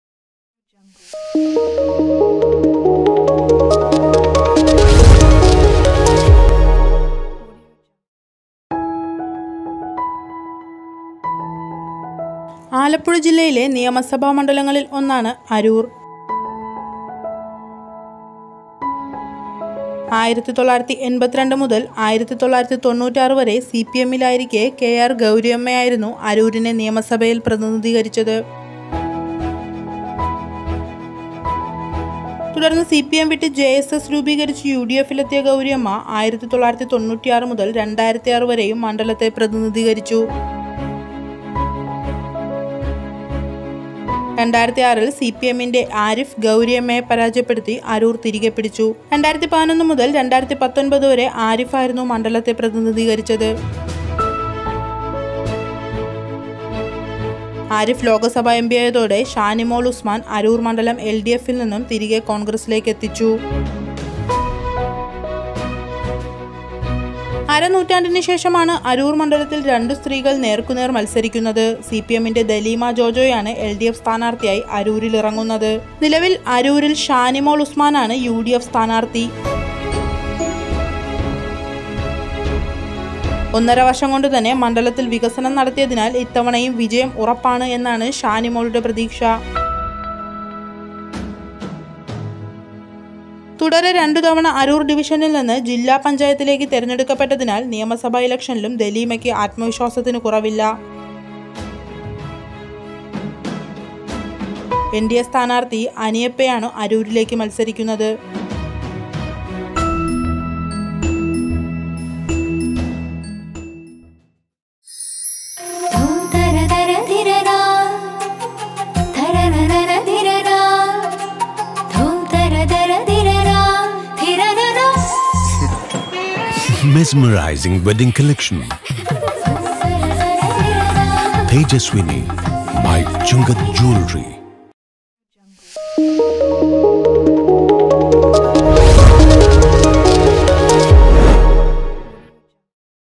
അരൂരിൽ ആര് | Anweshanam.com
ആലപ്പുഴ ജില്ലയിലെ നിയമസഭാ മണ്ഡലങ്ങളിൽ ഒന്നാണ് അരൂ‍ർ. 1982 മുതൽ 1996 വരെ സിപിഎമ്മിലായിരിക്കെ കെആ‍‍ർ ഗൗരിയമ്മയായിരുന്നു അരൂരിനെ നിയമസഭയിൽ പ്രതിനിധീകരിച്ചത്. തുട‍ർന്ന് സിപിഎം വിട്ട് ജെഎസ്എസ് രൂപീകരിച്ച് യുഡിഎഫിലെത്തിയ ഗൗരിയമ്മ 1996 മുതൽ 2006 വരെയും മണ്ഡലത്തെ പ്രതിനിധീകരിച്ചു. 2006ൽ സിപിഎമ്മിന്റെ എഎം ആരിഫ് ഗൗരിയമ്മയെ പരാജയപ്പെടുത്തി അരൂ‍ർ തിരികെ പിടിച്ചു. 2011 മുതൽ 2019 വരെ ആരിഫായിരുന്നു മണ്ഡലത്തെ പ്രതിനിധീകരിച്ചത്.